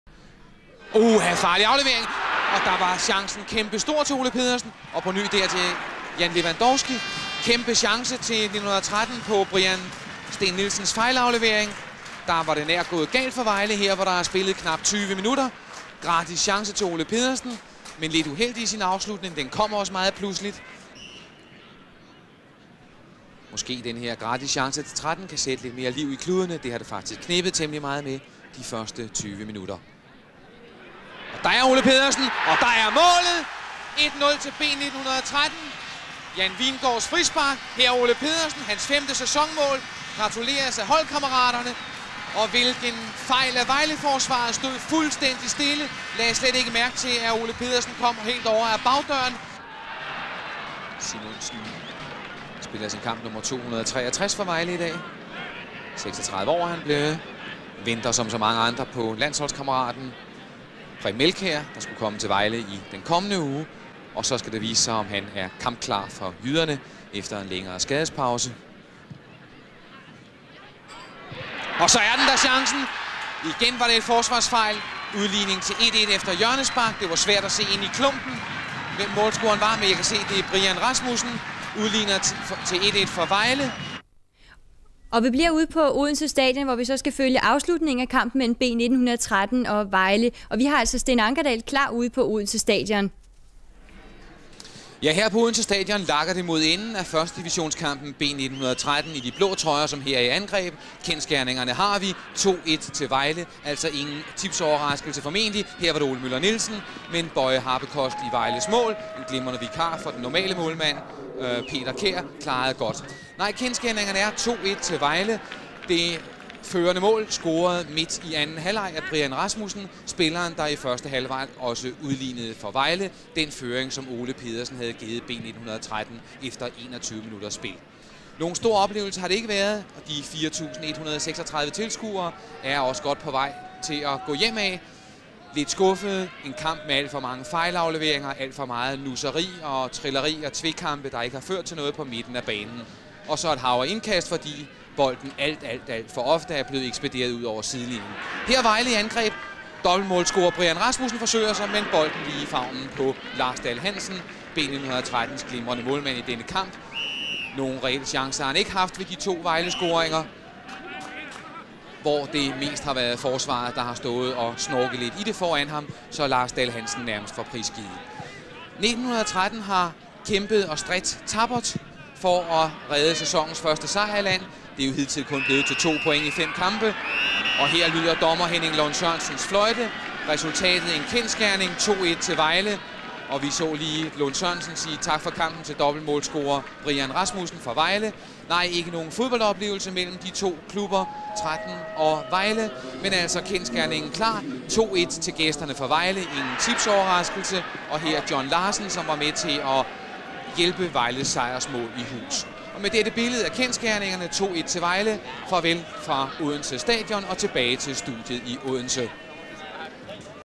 7. maj 1989 B 1913-Vejle Boldklub 1-2
B 1913-VB 1-2 (1-1)
Boye Habekost-Henrik Risom, John Larsen, Lars Rud, Brian Steen Nielsen - Johnny Mølby, Allan Simonsen, Carsten Pedersen - Jacob Svinggaard, Brian Rasmussen, Henrik Ravn

Udskiftningsspillere :
 Morten Bech (afløste i det 51, min. Allan Simonsen) og Flemming Andreasen (afløste i det 65, min. Henrik Ravn).

Kampens spiller : Boye Habekost

Kampens gang :
19. min. 1-0 Ole Pedersen.
25. min. 1-1 Brian Rasmussen.
69. min. 1-2 Brian Rasmussen.

Dommer : Henning Lund Sørensen
Tilskuere : 4.134

VB har det åbenbart med kun at spille godt hveranden gang. Efter den fornemme kamp ugen i forvejen blev det en halvtam affære mod nyoprykkerne B 1913 på Odense stadion. Udover målmand Boye Habekost og Brian Rasmussens to mål var der ikke de store ting at glæde sig over. Og set med retfærdighedens briller var det en heldig VB-sejr.
Fynboerne havde fortjent mindst det ene point.
At det kun blev til et enkelt fynsk mål, kan VB'erne først og fremmest takke reservemålmand Boye Habekost for. Han præsterede en lang række flotte redninger.
Boye Habekost måtte dog overgive sig, efter at Johnny Mølby havde lavet frispark i højre side. På en udadskruet bold blev VB-forsvaret fanget, og en udækket Ole Pedersen udnyttede omgående chancen.
Den fynske glæde varede kun fire minutter. På hjørnespark sendte Henrik Risom bolden ind foran mål, hvor den strejfede en fynsk forsvarer, inden den havnede hos Brian Rasmussen, som scorede via en fynsk fod.
Efter pausen kom der noget mere liv over VB's spil, og sejrsmålet faldt, efter at B 1913's målmand havde klaret et glimrende forsøg af Henrik Risom.
På det efterfølgende hjørnespark fik John Larsen bolden sendt videre til Brian Rasmussen, som scorede på et velplaceret hovedstød. Samme Brian Rasmussen var senere tæt ved at gøre det til 1-3, igen på hovedstød.
Mod kampens slutning var hjemmeholdet to gange tæt på at udligne, men det blev ved den ikke helt fortjente VB-sejr.